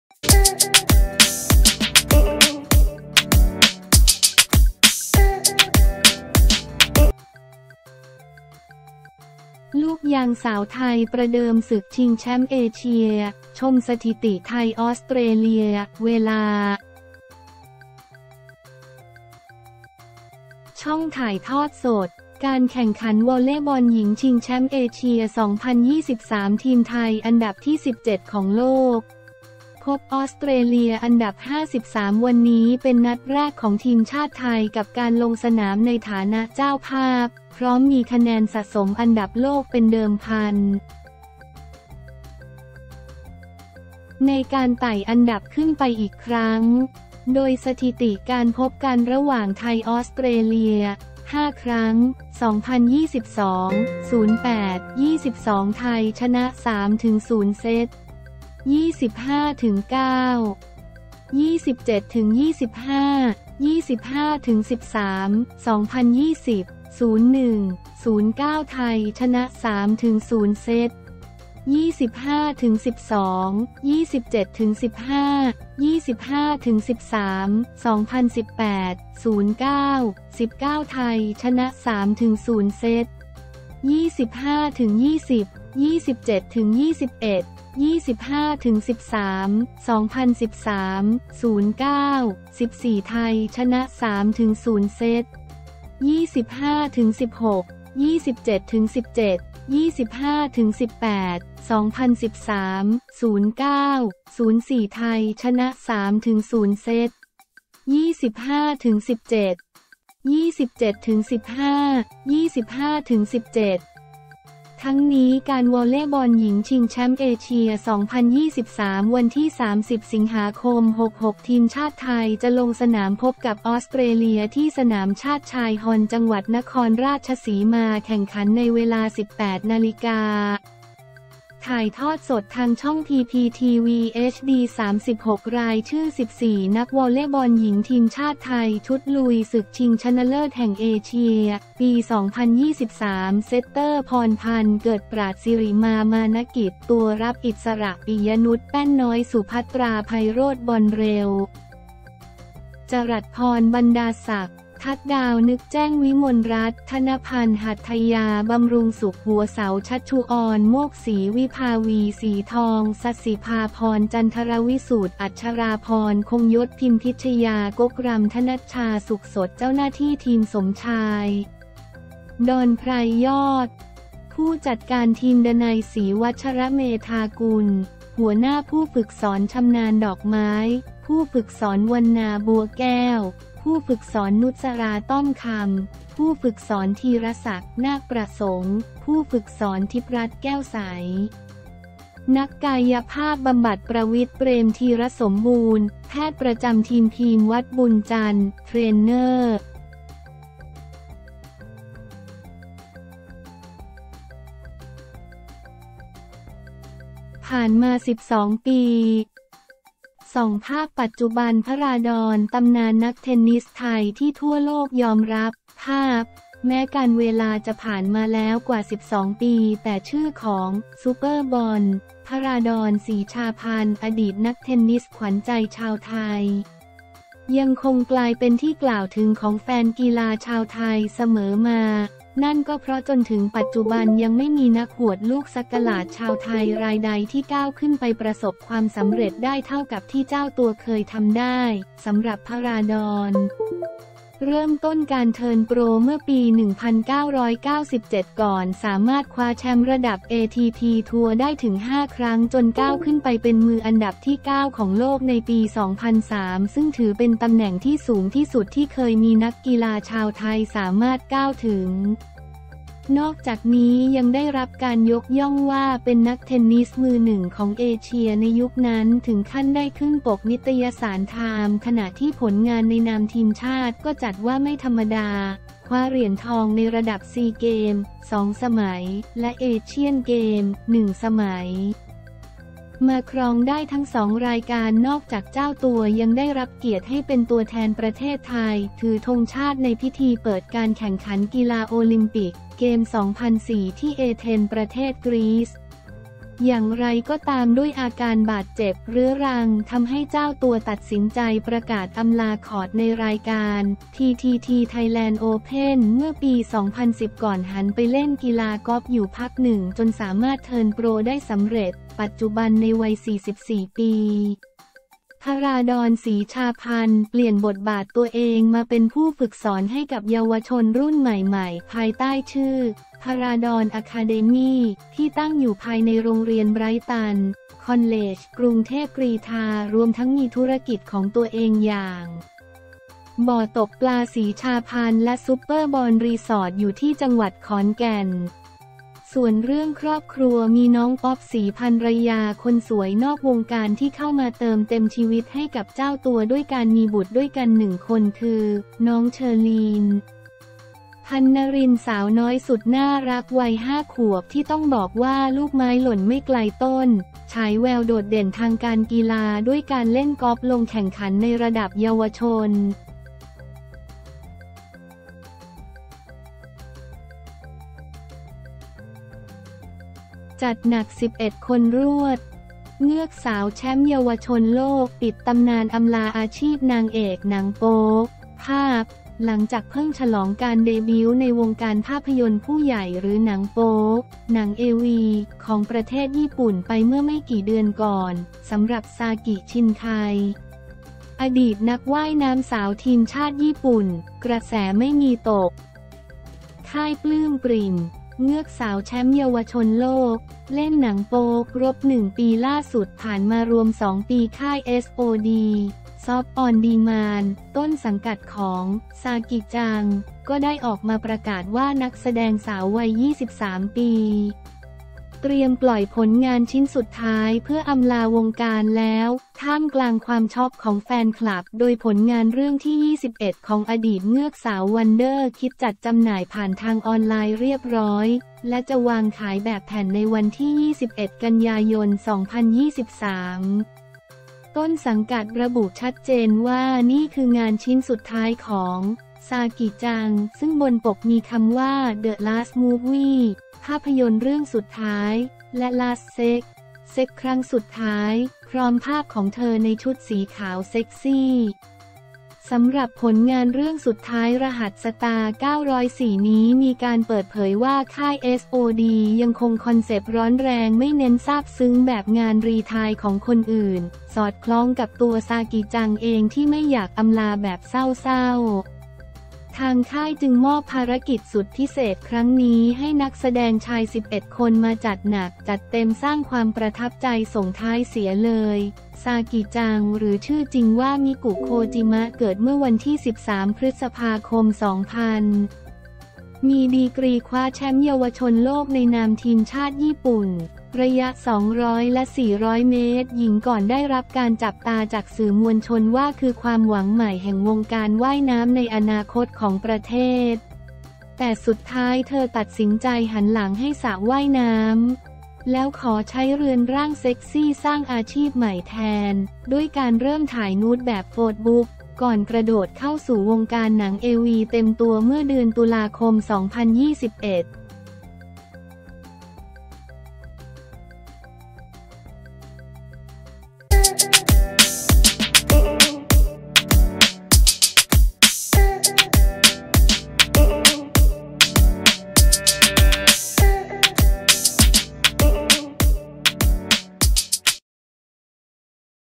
ลูกยางสาวไทยประเดิมศึกชิงแชมป์เอเชีย ชมสถิติ ไทย ออสเตรเลีย, เวลา, ช่องถ่ายทอดสด
การแข่งขันวอลเลย์บอลหญิงชิงแชมป์เอเชีย 2023 ทีมไทย อันดับที่ 17 ของโลก พบ ออสเตรเลีย อันดับ 53 วันนี้เป็นนัดแรกของทีมชาติไทยกับการลงสนามในฐานะเจ้าภาพ พร้อมมีคะแนนสะสมอันดับโลกเป็นเดิมพัน ในการไต่อันดับขึ้นไปอีกครั้ง
โดย สถิติการพบกันระหว่าง ไทย - ออสเตรเลีย (5 ครั้ง)

2022/08/22 ไทย ชนะ 3-0 เซต (25-9, 27-25, 25-13)

2020/01/09 ไทย ชนะ 3-0 เซต (25-12, 27-15, 25-13)

2018/09/19 ไทย ชนะ 3-0 เซต (25-20, 27-21, 25-13)

2013/09/14 ไทย ชนะ 3-0 เซต (25-16, 27-17, 25-18)

2013/09/04 ไทย ชนะ 3-0 เซต (25-17, 27-15, 25-17)

ทั้งนี้การวอลเลย์บอลหญิงชิงแชมป์เอเชีย 2023 วันที่ 30 สิงหาคม 66  ทีมชาติไทย จะลงสนามพบกับ ออสเตรเลีย ที่ สนามชาติชาย ฮอลล์ จ.นครราชสีมา
แข่งขันในเวลา 18.00 น. ถ่ายทอดสดทางช่อง PPTV HD36

รายชื่อ 14 นักวอลเลย์บอลหญิงทีมชาติไทย ชุดลุยศึกชิงชนะเลิศแห่งเอเชีย ปี 2023
เซตเตอร์
พรพรรณ เกิดปราชญ์
สิริมา มานะกิจ

ตัวรับอิสระ
ปิยะนุช แป้นน้อย
สุพัตรา ไพโรจน์
บอลเร็ว
จรัสพร บรรดาศักดิ์
ทัดดาว นึกแจ้ง
วิมลรัตน์ ทะนะพันธุ์
หัตถยา บำรุงสุข

หัวเสา
ชัชชุอร โมกศรี
วิภาวี ศรีทอง
ศศิภาพร จันทรวิสูตร
อัจฉราพร คงยศ
พิมพิชยา ก๊กรัมย์
ธนัชชา สุขสด

เจ้าหน้าที่ทีม
สมชาย ดอนไพรยอด : ผู้จัดการทีม
ดนัย ศรีวัชรเมธากุล : หัวหน้าผู้ฝึกสอน
ชำนาญ ดอกไม้ : ผู้ฝึกสอน
วรรณา บัวแก้ว : ผู้ฝึกสอน
นุศรา ต้อมคำ : ผู้ฝึกสอน
ธีรศักดิ์ นาคประสงค์ : ผู้ฝึกสอน
ทิพย์รัตน์ แก้วใส : นักกายภาพบำบัด
ประวิทย์ เปรมธีรสมบูรณ์ : แพทย์ประจำทีม
ภีมวัจน์ บุญจันทร์ : เทรนเนอร์